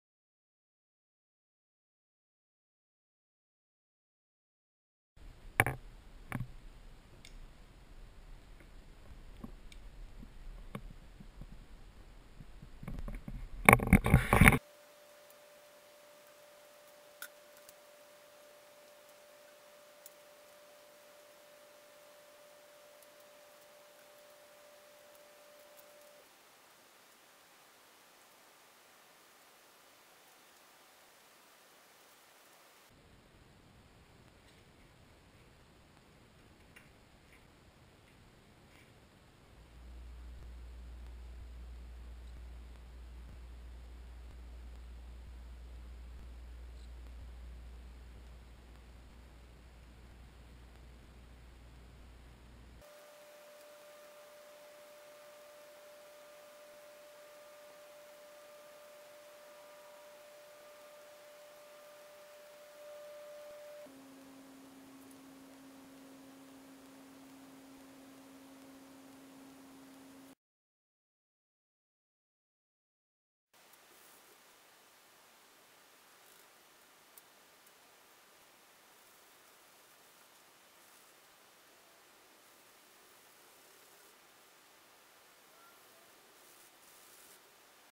Wakeboard SunCure, HOW TO
Quick demonstration on how to repair a damaged wakeboard.